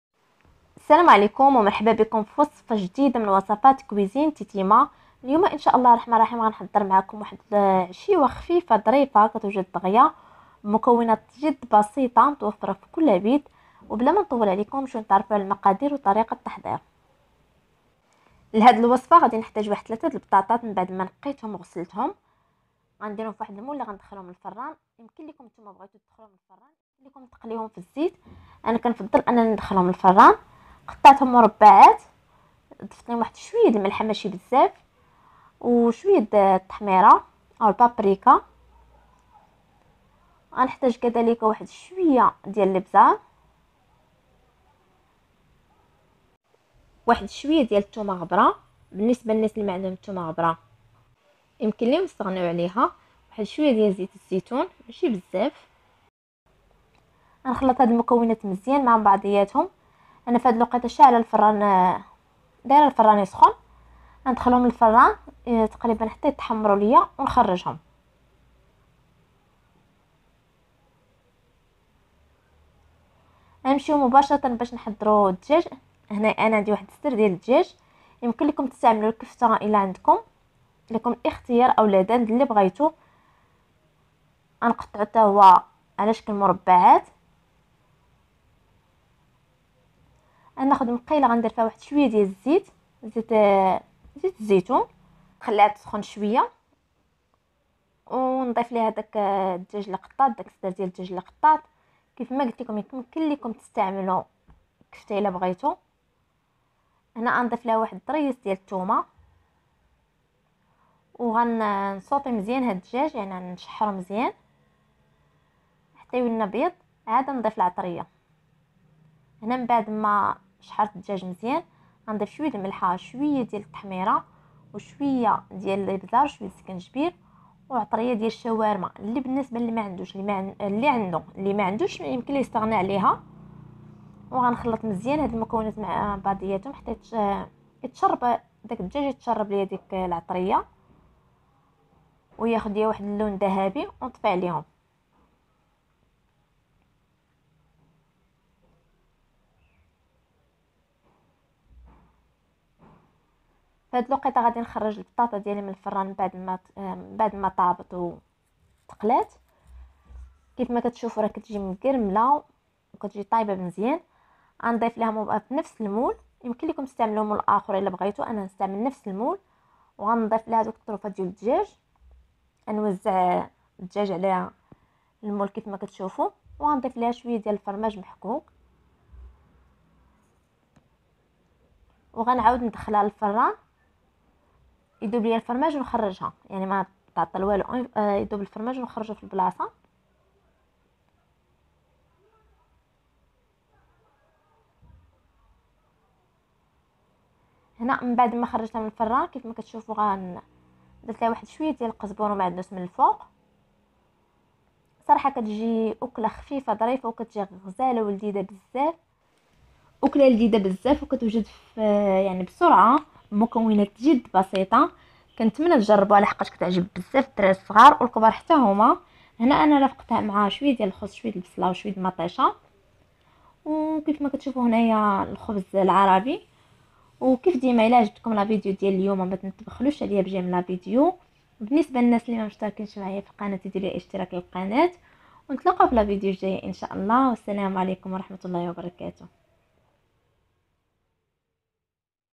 وجبة عشاء خفيفة وسريعة توجد في دقائق ب3 مكونات فقط😋😋😋
وجبة عشاء خفيفة وسريعة توجد بسرعة بمكونات بسيطة جدا